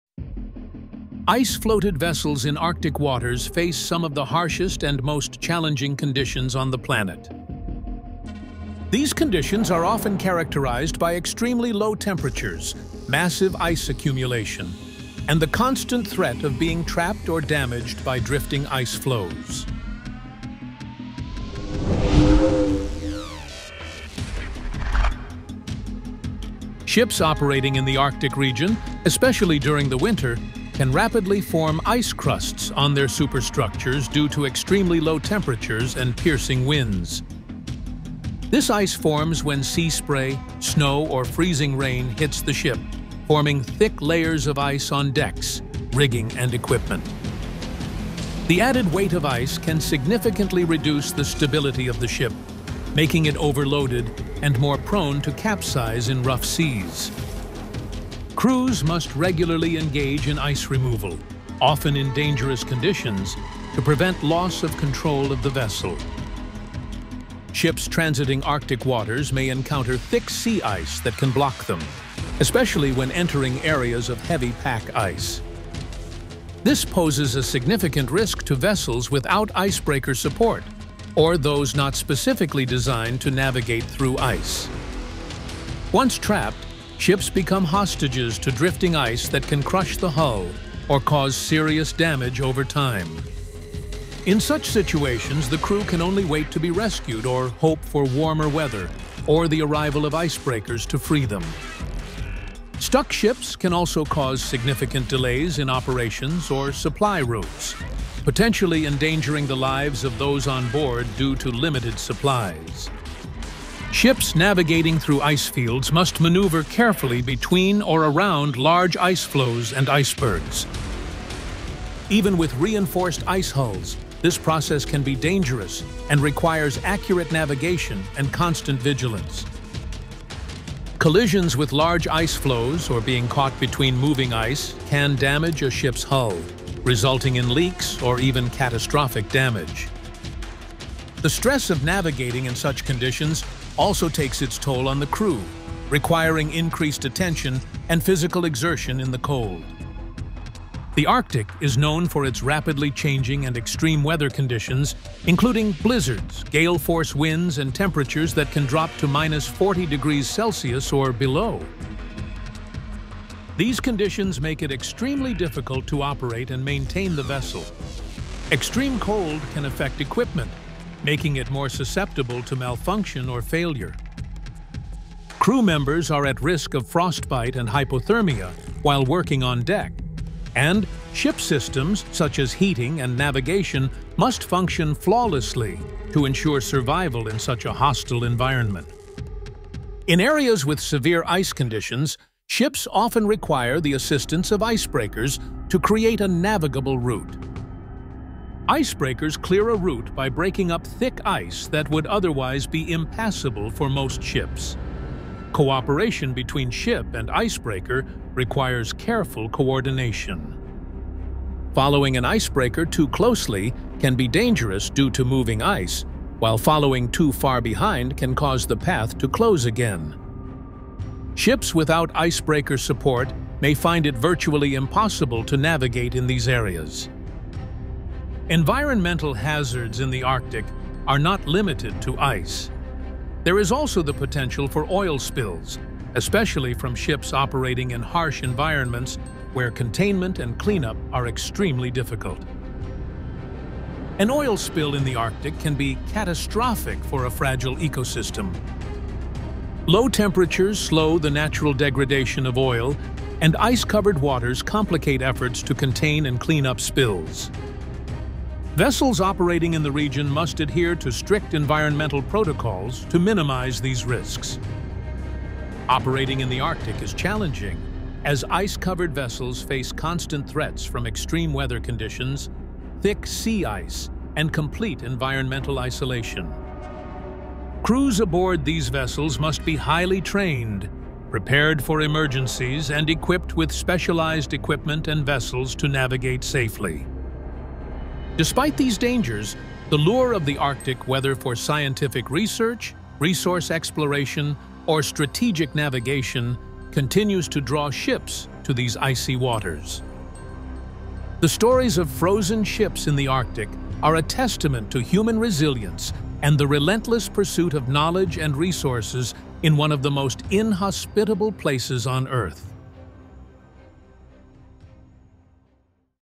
Arctic Ice Ships: Overcoming Extreme Conditions in the Coldest Region on Earth
Episodes:
0:00 - Ice-covered ships in the Arctic
0:25 - Operating in the Arctic region
1:07 - Ice captivity of a ship in the Arctic
1:55 - Navigating ships through ice fields
2:31 - Extreme weather conditions in the Arctic during ship navigation
3:20 - Interaction with icebreakers in the Arctic
3:50 - Risk of environmental hazards in the Arctic

Ice-floated vessels in Arctic waters face some of the harshest and most challenging conditions on the planet.
These conditions are often characterized by extremely low temperatures, massive ice accumulation, and the constant threat of being trapped or damaged by drifting ice floes.